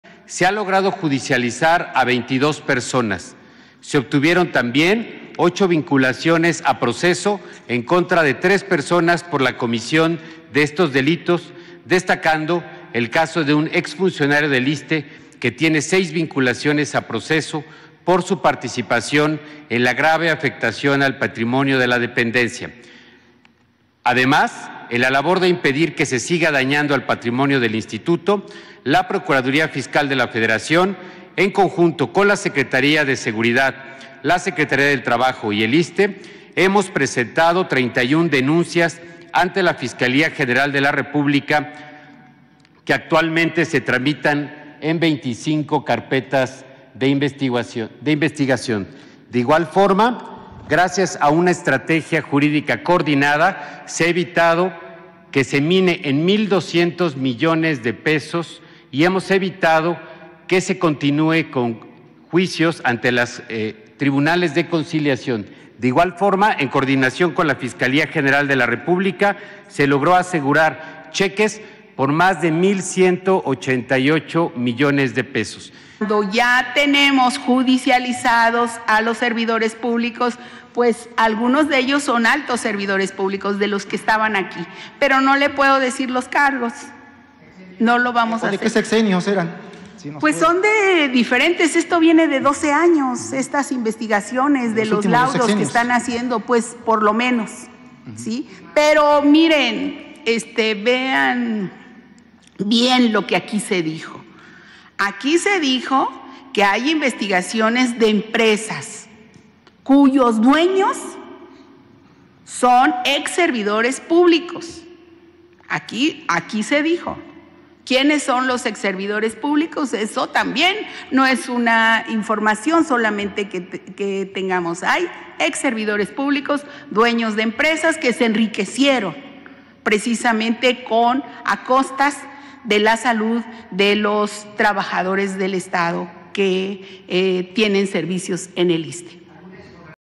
Se ha judicializado a 22 personas por saqueo y fraude al ISSSTE, incluyendo a ex servidores públicos
Se ha judicializado a 22 personas por saqueo y fraude al ISSSTE, incluyendo a ex servidores públicos de altos mandos que figuran como dueños de empresas que se enriquecieron con la institución